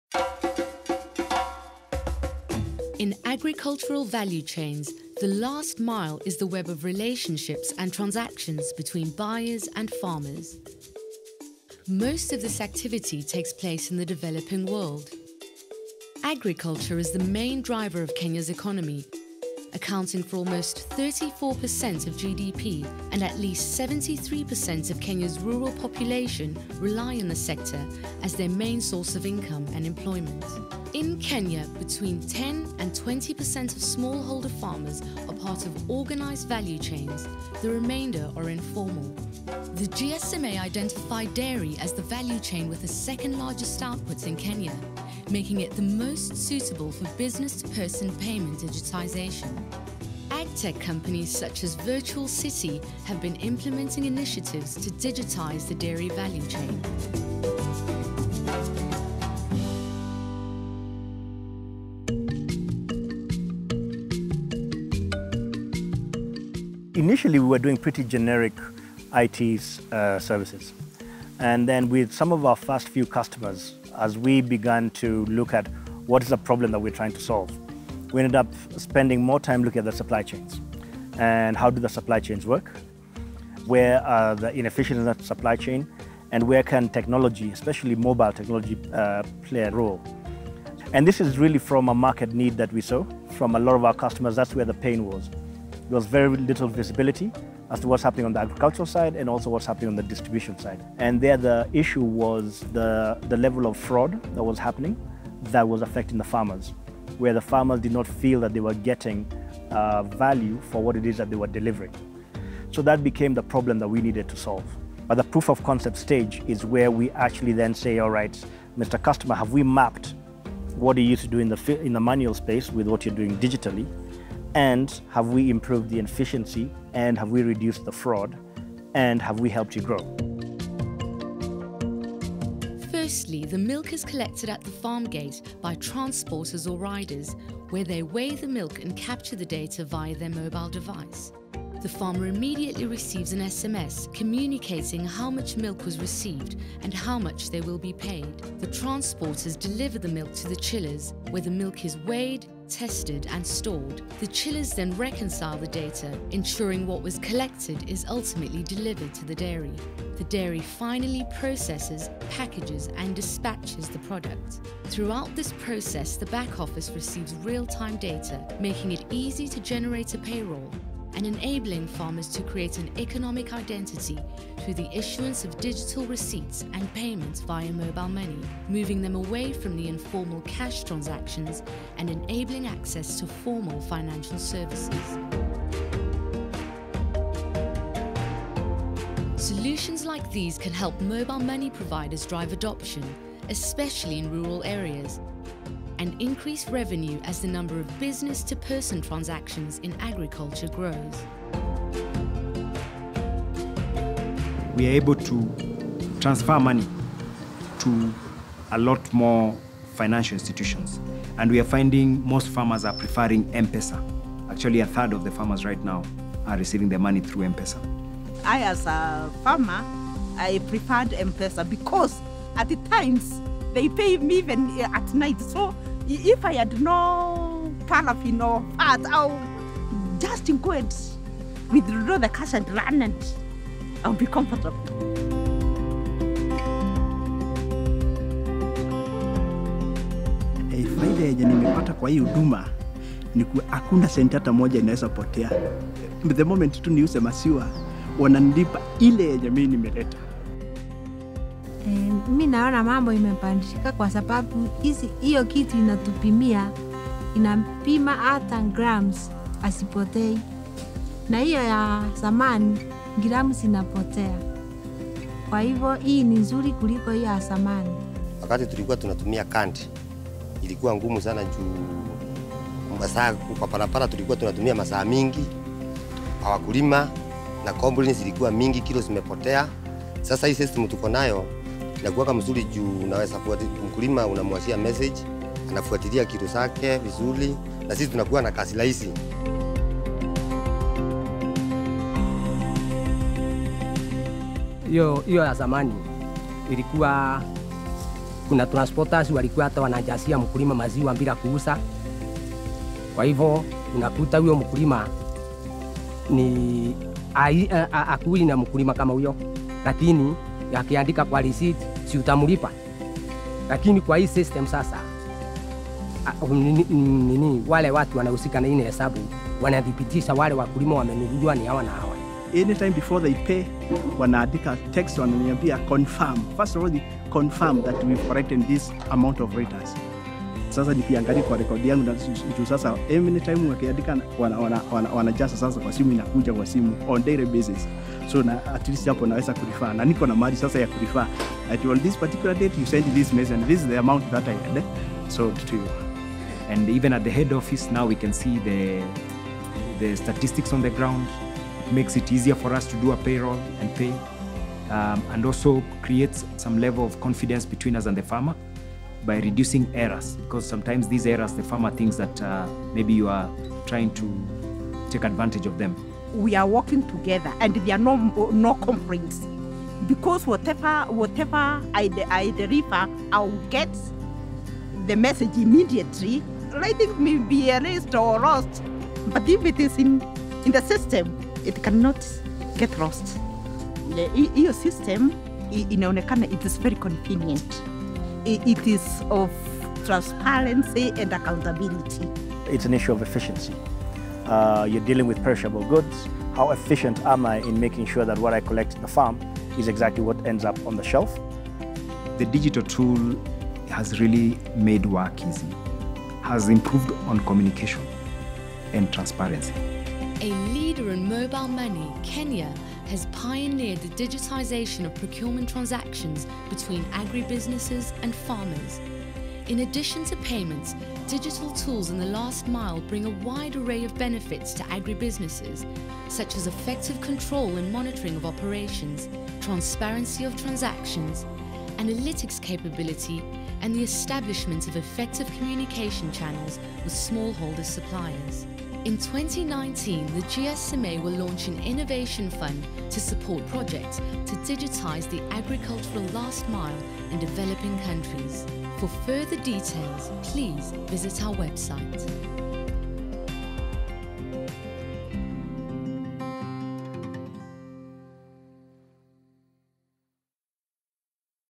Bringing efficiency to Kenya’s dairy value chain: Virtual City’s Agrimanagr
Agrimanagr is a digital tool created by Virtual City, a Kenyan AgTech company. The tool enables real-time visibility in the last mile of the agricultural value chain. Agrimanagr digitises the entire procurement process, benefiting both buyers, such as agribusinesses and cooperatives, and smallholder farmer suppliers.

The mobile-based tool brings about accuracy in weighing, in recording and tracking produce, and in farmer data – increasing efficiencies in the agricultural last mile. Importantly, the integration of mobile money payments with the tool will enable Agrimanagr to support the creation of an economic identity for farmers, giving them an opportunity to access formal financial services.